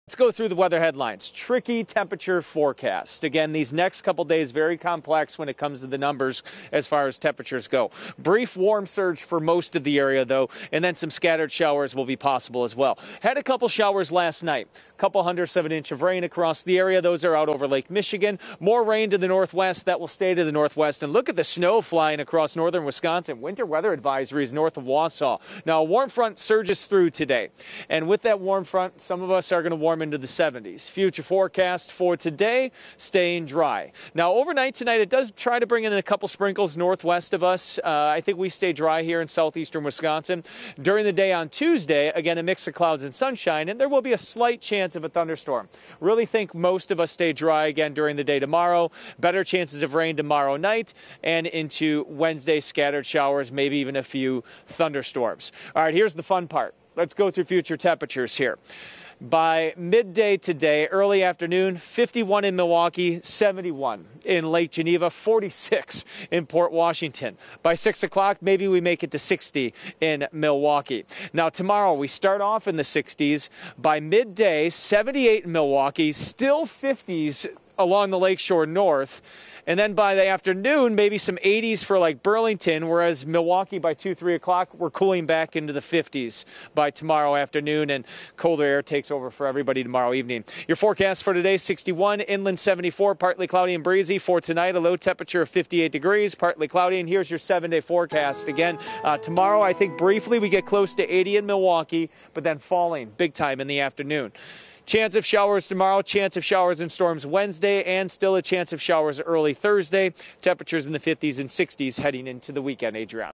Wide range of temperatures expected as warm front moves through
A warm front will move through, and behind the front temperatures will reach the 70s this afternoon away from the lake.